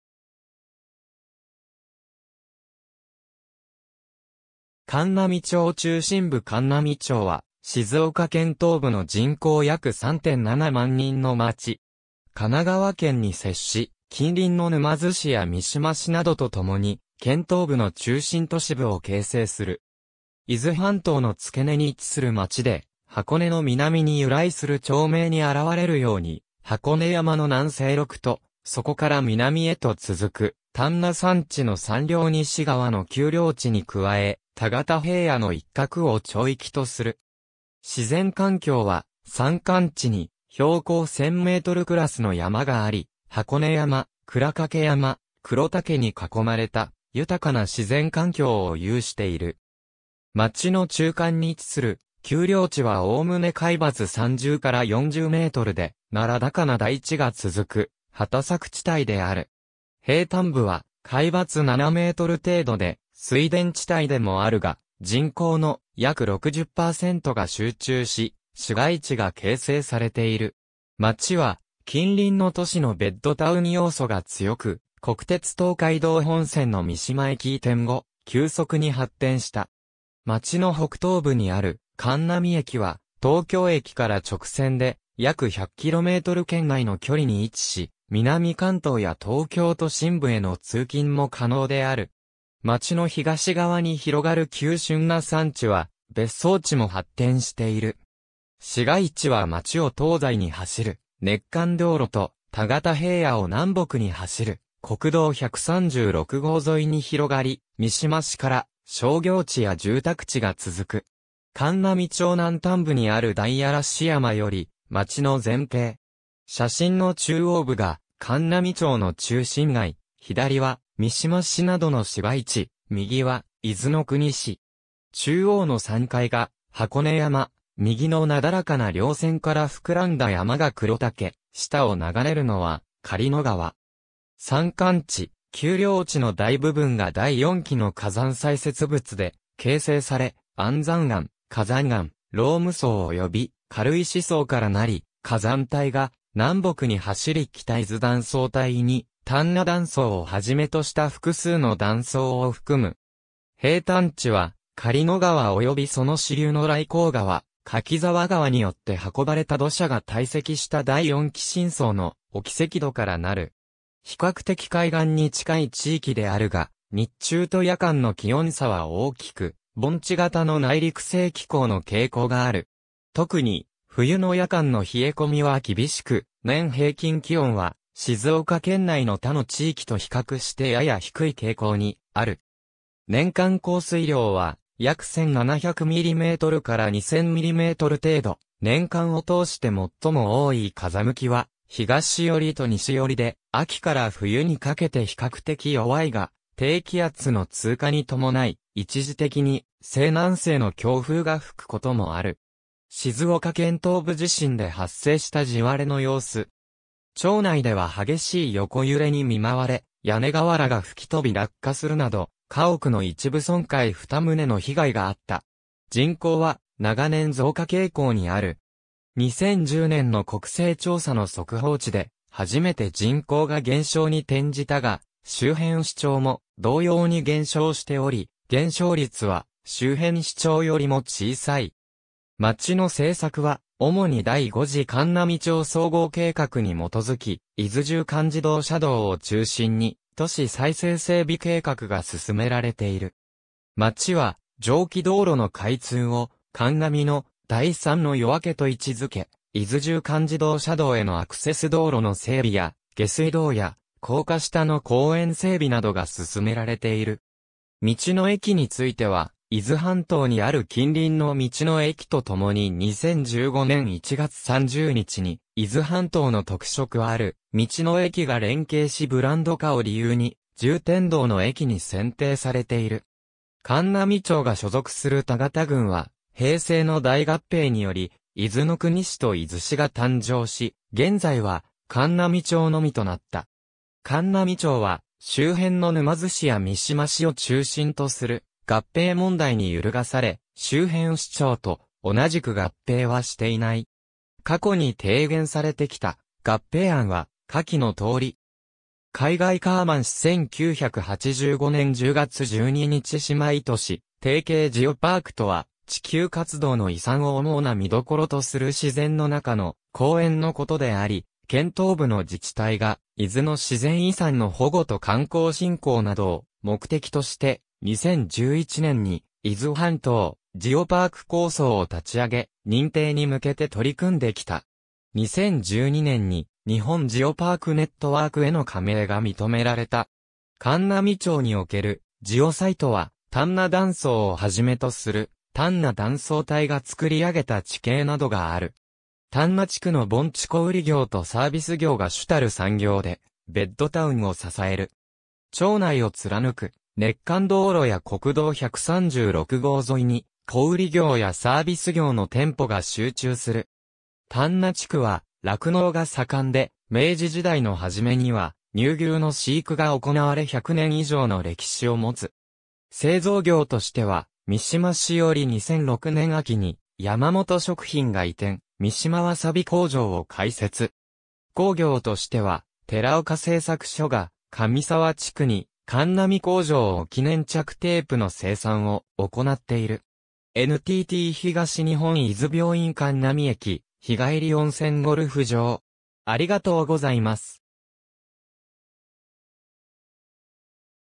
函南町
函南町中心部 函南町（かんなみちょう）は静岡県東部の人口約3.7万人の町。
神奈川県に接し、近隣の沼津市や三島市などとともに、県東部の中心都市部を形成する。
伊豆半島の付け根に位置する町で、箱根（別名：函嶺）の南に由来する町名に表れるように、箱根山の南西麓と、そこから南へと続く丹那山地の山稜西側の丘陵地に加え、田方平野の一角を町域とする。
自然環境は、山間地に標高1000mクラスの山があり、箱根山、鞍掛山、玄岳に囲まれた豊かな自然環境を有している。
町の中間に位置する丘陵地は概ね海抜30〜40mで、ならだかな台地がつづく畑作地帯である。
平坦部は海抜7m程度で、水田地帯でもあるが、人口の約60%が集中し、市街地が形成されている。
町は近隣の都市のベッドタウン要素が強く、国鉄東海道本線の三島駅移転後、急速に発展した。
町の北東部にある函南駅は東京駅から直線で約100km圏内の距離に位置し、南関東や東京都心部への通勤も可能である。
町の東側に広がる急峻な山地は別荘地も発展している。
市街地は町を東西に走る熱函道路と、田方平野を南北に走る国道136号沿いに広がり、三島市から商業地や住宅地が続く。
函南町南端部にある大嵐山（日守山）より町の全景。
写真の中央部が函南町の中心街、左は三島市などの市街地、右は伊豆の国市。
中央の山塊が箱根山、右のなだらかな稜線から膨らんだ山が玄岳、下を流れるのは狩野川。
山間地、丘陵地の大部分が第四紀の火山砕屑物で形成され、安山岩、火山岩、ローム層及び軽石層からなり、火山帯が南北に走り北伊豆断層帯に丹那断層をはじめとした複数の断層を含む。
平坦地は、狩野川及びその支流の来光川、柿沢川によって運ばれた土砂が堆積した第四紀新層の沖積土からなる。
比較的海岸に近い地域であるが、日中と夜間の気温差は大きく、盆地型の内陸性気候の傾向がある。
特に、冬の夜間の冷え込みは厳しく、年平均気温は静岡県内の他の地域と比較してやや低い傾向にある。
年間降水量は、約1700mm〜2000mm程度、年間を通して最も多い風向きは東寄りと西寄りで、秋から冬にかけて比較的弱いが、低気圧の通過に伴い、一時的に西南西の強風が吹く事もある。
静岡県東部地震（震度4）で発生した地割れの様子。
町内では激しい横揺れに見舞われ、屋根瓦が吹き飛び落下するなど家屋の一部損壊2棟の被害があった。
人口は長年増加傾向にある。
2010年（平成22年）の国勢調査の速報値で初めて人口が減少に転じたが、周辺市町も同様に減少しており、減少率は周辺市町よりも小さい。
町の政策は、主に第5次函南町総合計画（後期計画：平成24年度から平成28年度）に基づき、伊豆縦貫自動車道（東駿河湾環状道路）を中心に都市再生整備計画が進められている。
町は上記道路の開通を「函南の第三の夜明け」と位置付け、伊豆縦貫自動車道（東駿河湾環状道路）へのアクセス道路の整備や下水道や高架下の公園整備などが進められている。
道の駅については伊豆半島にある近隣の道の駅とともに2015年（平成27年）1月30日に「伊豆半島の特色ある「道の駅」が連携しブランド化」を理由に重点道の駅に選定されている。
函南町が所属する田方郡は平成の大合併により伊豆の国市と伊豆市が誕生し、現在は函南町のみとなった。
函南町は周辺の沼津市や三島市を中心とする合併問題（静岡県東部政令指定都市構想など）に揺るがされ、周辺市町と同じく合併はしていない。
過去に提言されてきた合併案は下記のとおり。
海外 カーマン市（アメリカ合衆国 カリフォルニア州）1985年10月12日姉妹都市提携 ジオパークとは、「地球活動の遺産を主な見所とする自然の中の公園」のことであり、県東部の自治体が、伊豆の自然遺産の保護と観光振興などを目的として、2011年に伊豆半島ジオパーク構想を立ち上げ、認定に向けて取り組んできた。
2012年に日本ジオパークネットワークへの加盟が認められた。
函南町におけるジオサイトは、 丹那断層をはじめとする丹那断層帯が創り上げた地形などがある。
丹那地区の盆地 小売業とサービス業が主たる産業でベッドタウンを支える。
町内を貫く熱函道路や国道136号沿いに、小売業やサービス業の店舗が集中する。
丹那地区は酪農が盛んで明治時代の初めには乳牛の飼育が行われ100年以上の歴史を持つ。
製造業としては三島市より2006年秋に山本食品が移転、三島わさび工場を開設。
工業としては寺岡製作所が上沢地区に函南工場を置き粘着テー...